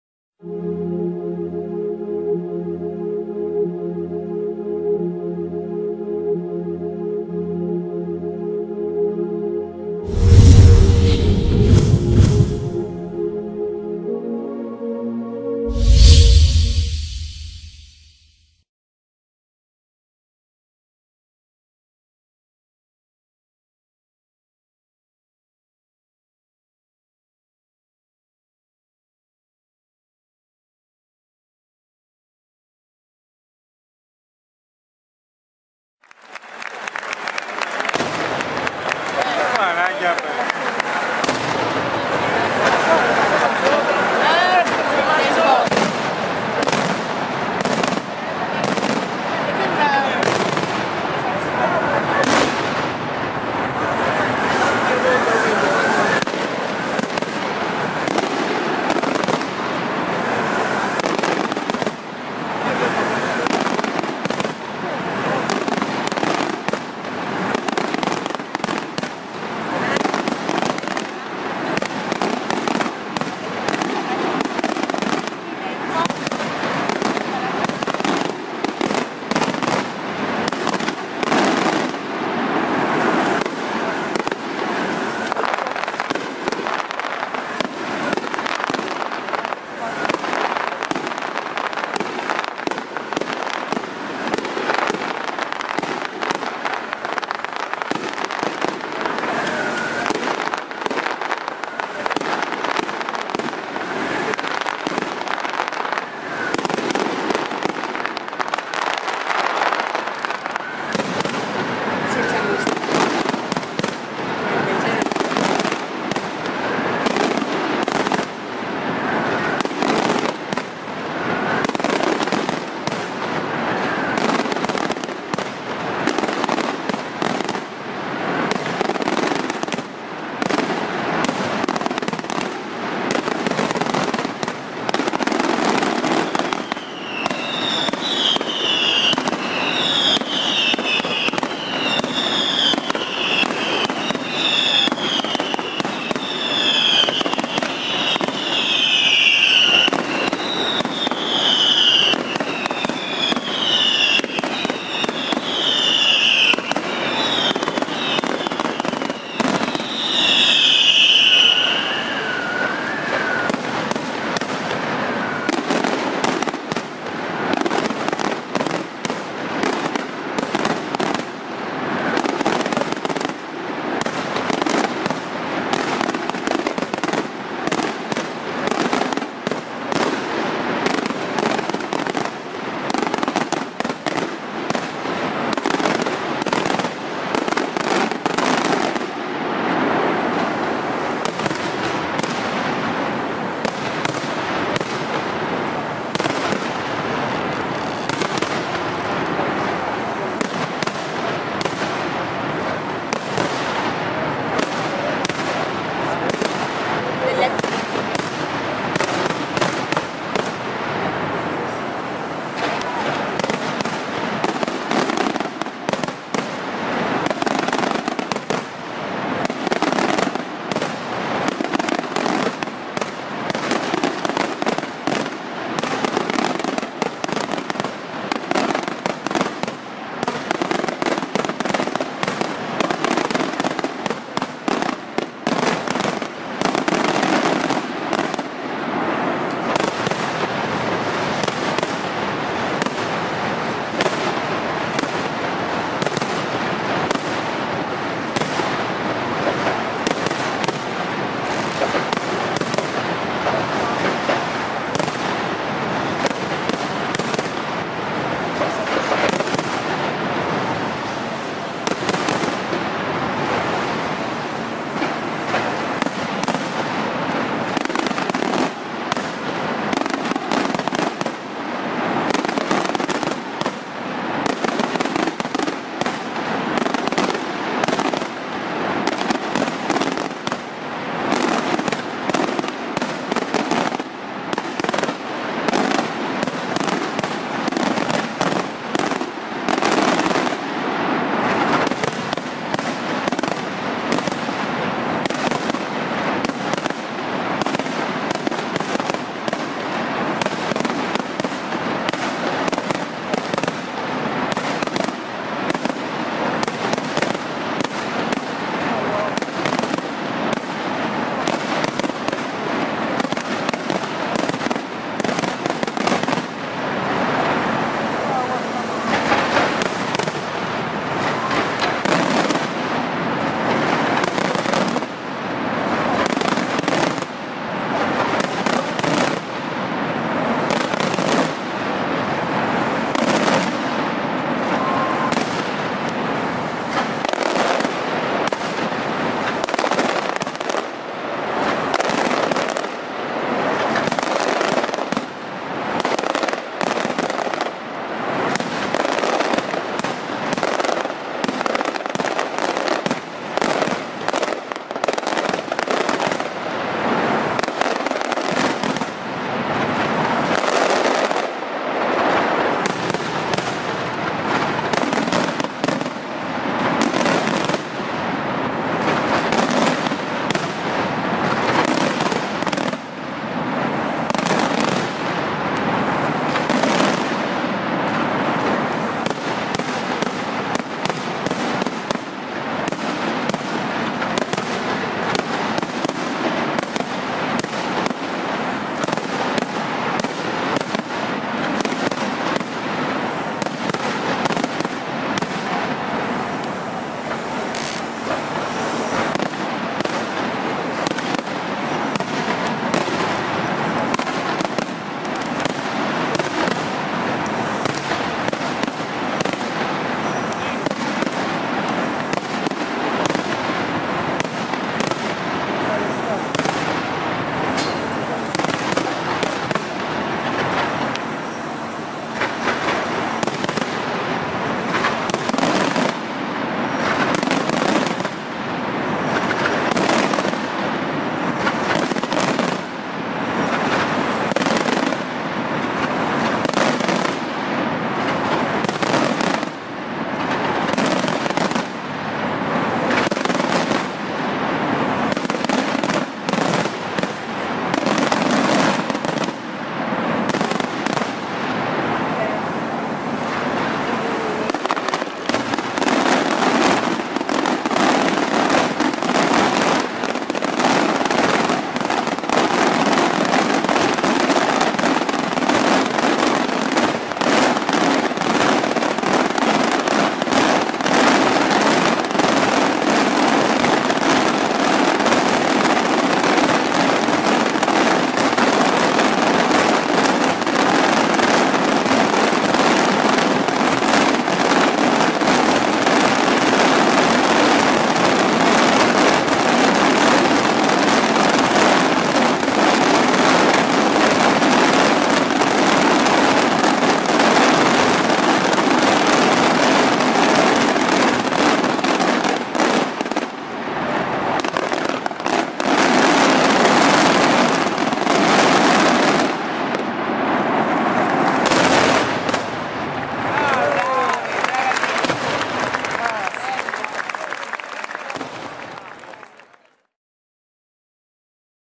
CATANIA - SANT'AGATA V.M. 2018 - VACCALLUZZO EVENTS (Notturno)
Catania - 3 Febbraio 2018
Festa di Sant'Agata Vergine e Martire

Spettacolo Pirotecnico al porto
VACCALLUZZO EVENTS da Belpasso (Ct)